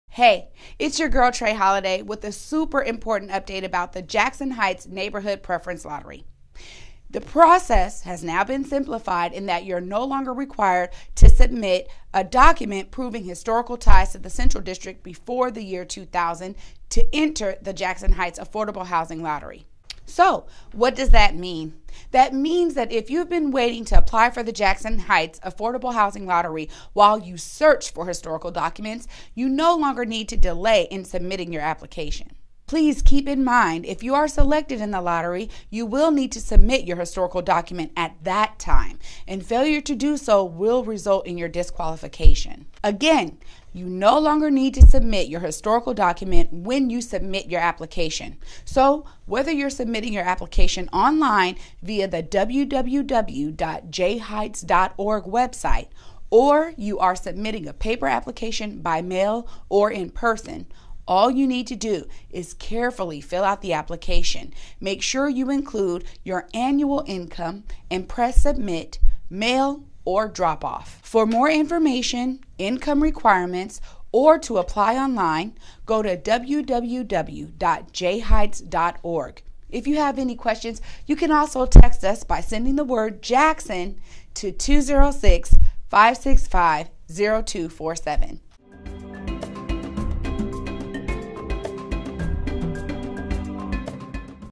ALERT ### JACKSON HEIGHTS NEW SIMPLIFIED APPLICATION ### ALERT
ALERT - The application process has now been simplified in that you are no longer required to submit a document proving historical ties to the Central District before the year 2000 prior to entering the Jackson Heights Affordable Housing Lottery.

So what does that mean? That means that if you have been waiting to apply for the Jackson Heights Affordable Housing Lottery while you searched for historical documents, you no longer need to delay in submitting your application.

Please keep in mind that If you are selected in the lottery, you will need to submit your historical document at that time and failure to do so will result in your disqualification.

Again, you no longer need to submit your historical document when you submit your application. So whether you are submitting your application online via the website or you are submitting a paper application by mail or in person. All you need to do is carefully fill out the application, make sure you include your annual income, and press submit, mail or drop off.